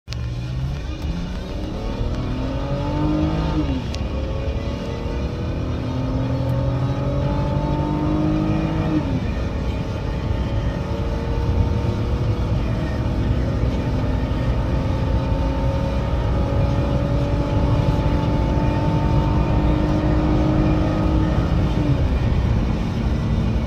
2011 Land Rover lr2 Acceleration
3.2L straight 6. 232 horsepower. Intelligent 4x4. Around 4500lbs.